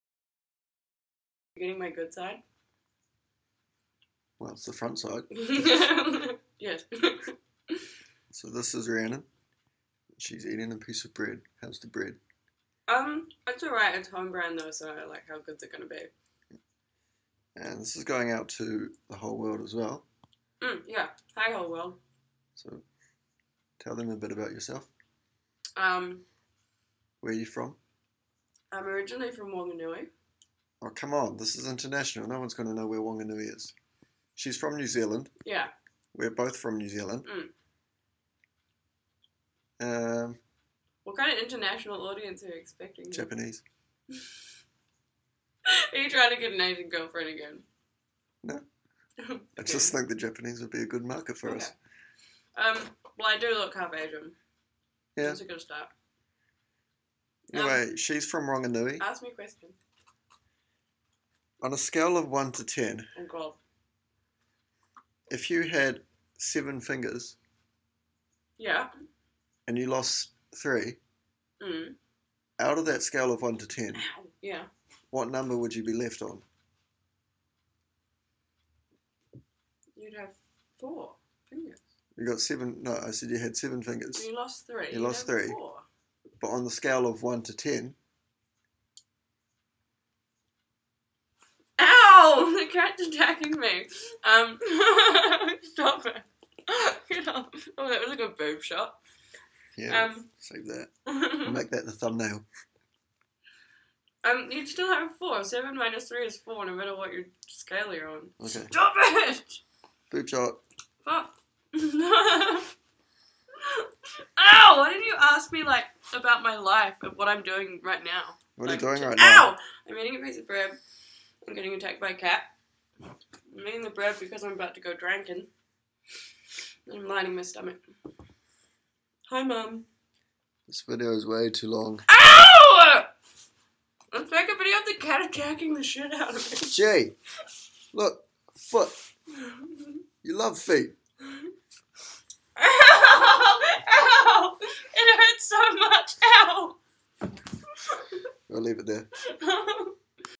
Hot Lady from NZ eats Bread and talks with mouth about not so interesting things
Bread is what she eats in NZ for chewing mouth energy.